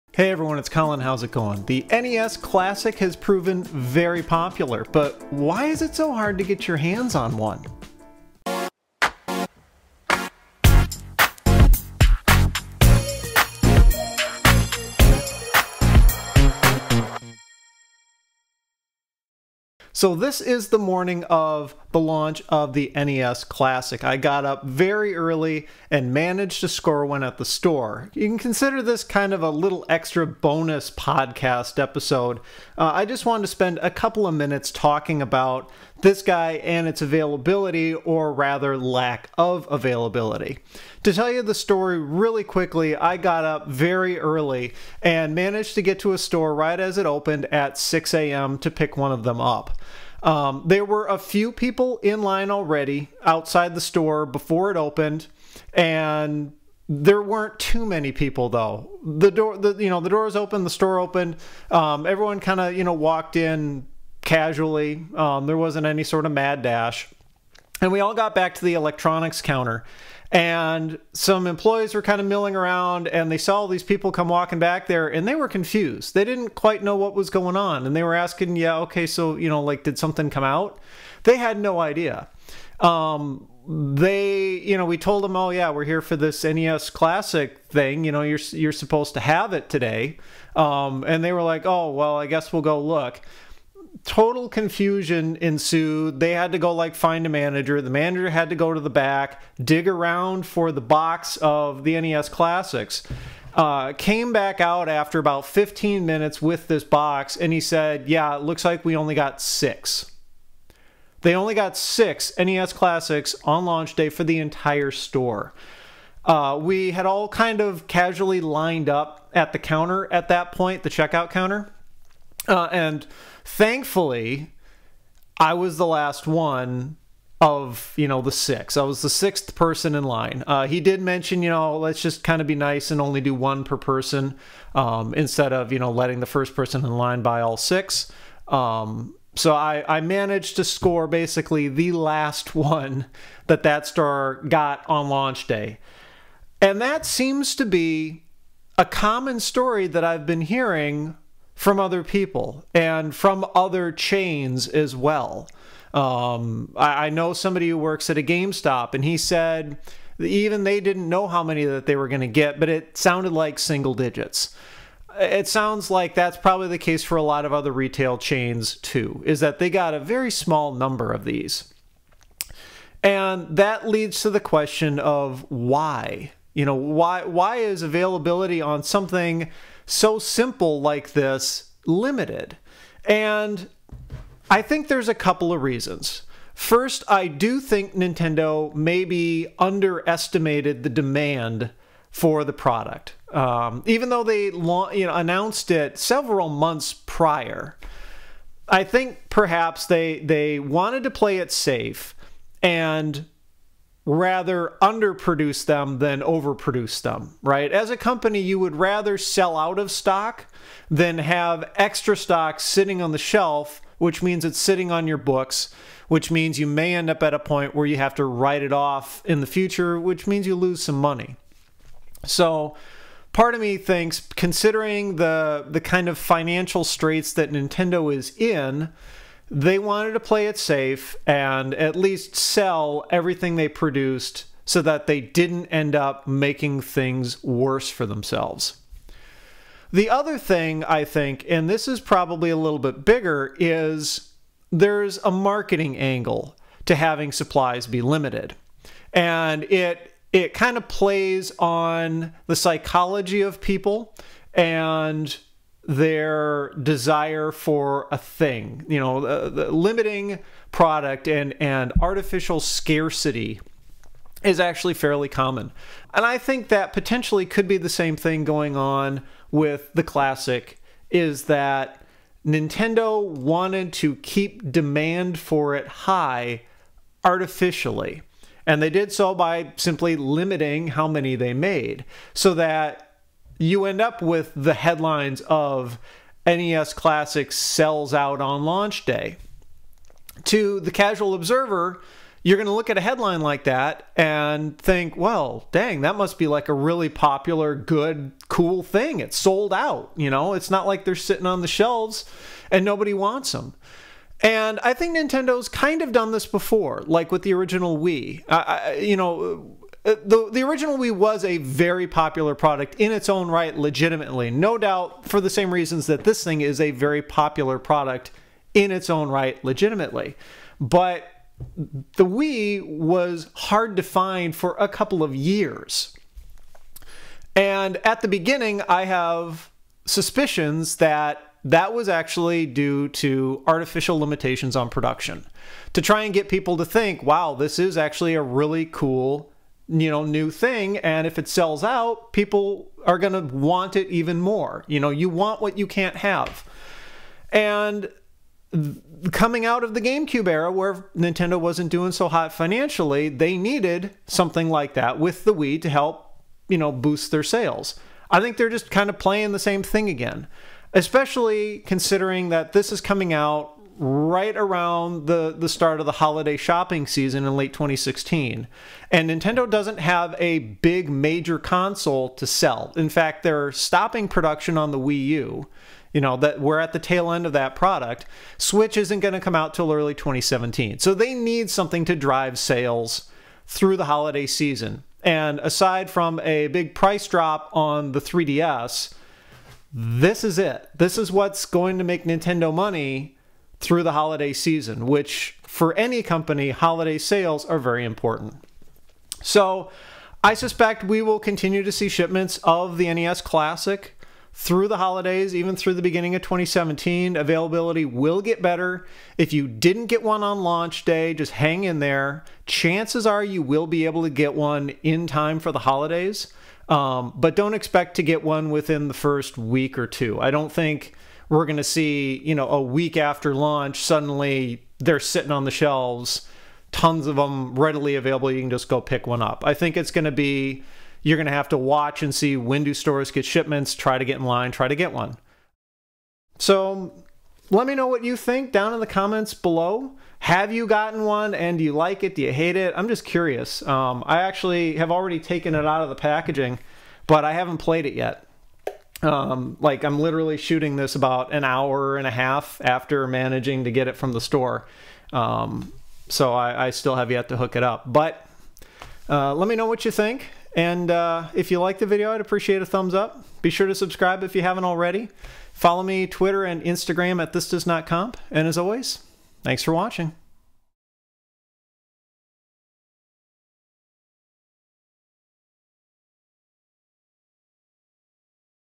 Why is the NES Classic Hard to Find?
Nintendo's new NES Classic retro console pretty much sold out on launch day, and will likely be hard to find for a while. But why are there so few to go around?